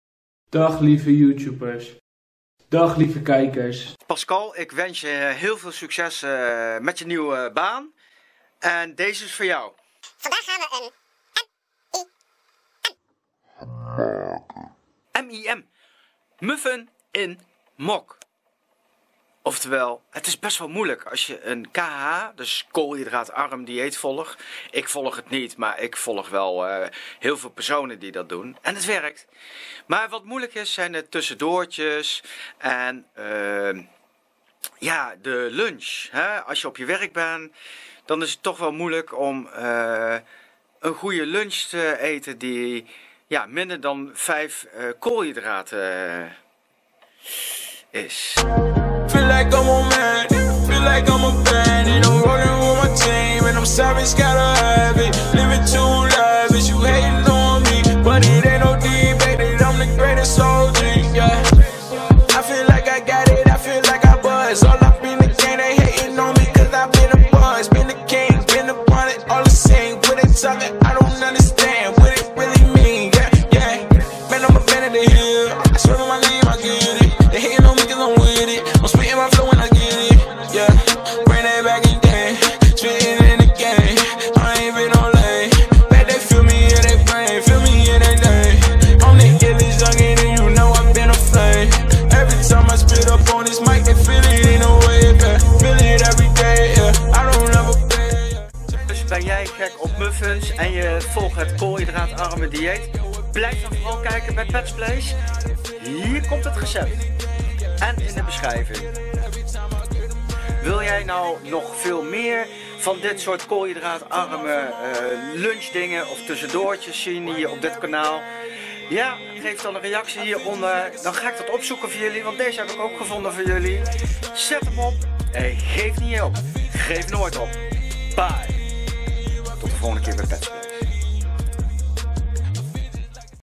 LOW CARB MUFFIN IN MUG
Low carb muffin in mug = 4kh
1 egg, 1 spoon 'mascapone' , 30 gr almond flour, 1 spoon sesame zeed, 1 spoon broken linzeed, thea spoon baking soda, salt, 15gr mushrooms, 10 gr olive,
3 minutes in microwave, 750 watt
jammy, lets eat!!